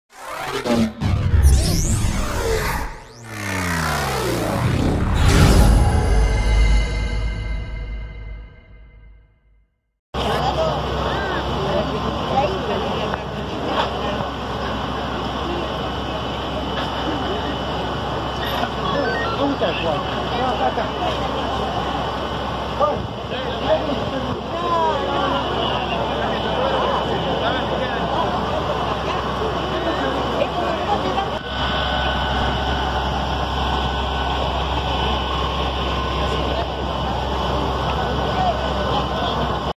Barrio Ituzaingó: importantes pérdidas en el incendio de una vivienda
Se registró minutos después de las 20 de este lunes. El fuego afectó a una propiedad ubicada en Azopardo y San Lorenzo. Un gran operativo policial y de Bomberos Voluntarios se montó para controlar la situación.